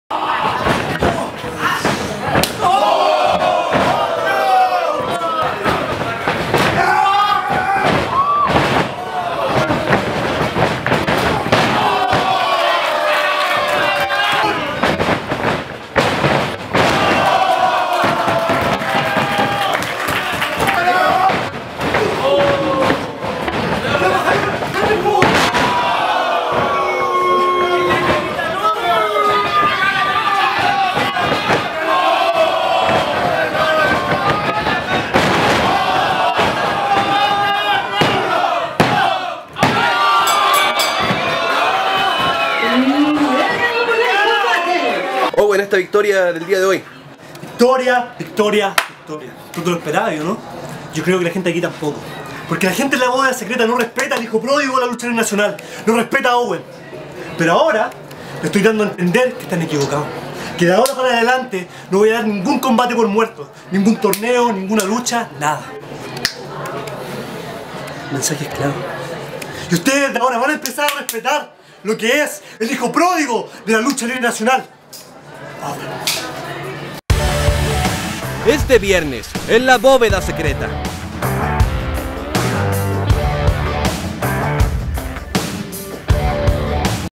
Carnicero Vs. Owen ¿Te lo esperabas?
Owen se llevo tremenda victoria el pasado Viernes frente a "El hombre del Matadero Franklin" Carnicero.

Hoy Owen estará enfrentando al Rey Clandestino 2019; Mr. Keyton.